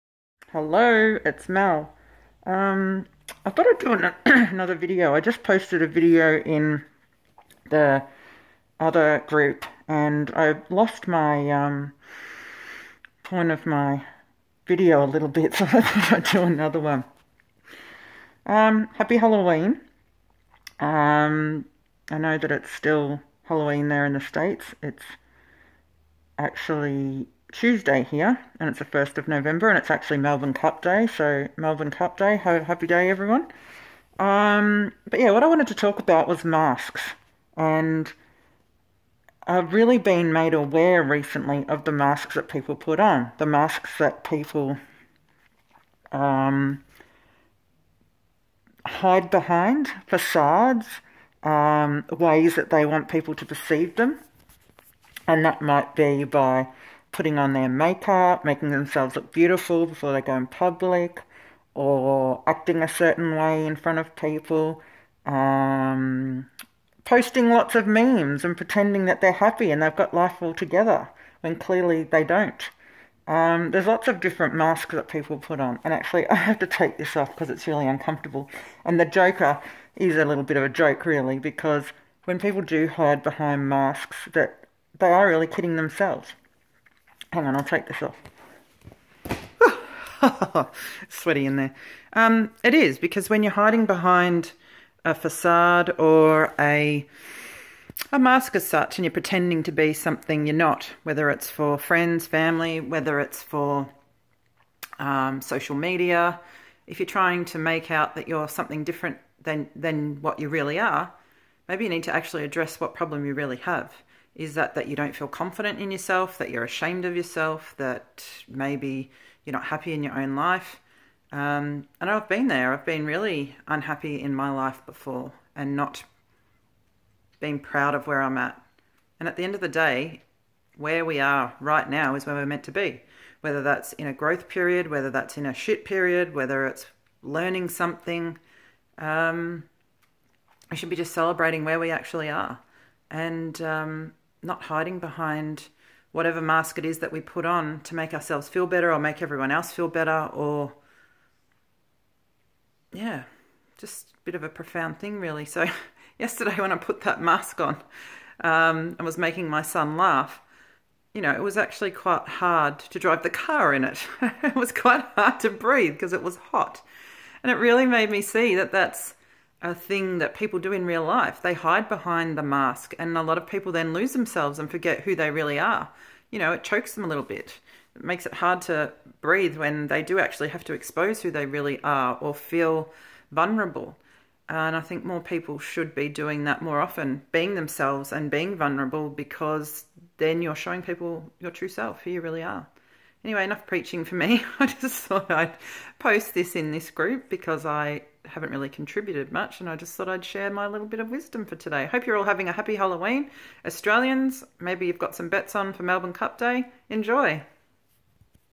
Take off the mask
Why you should be authentic and not put up a facade/mask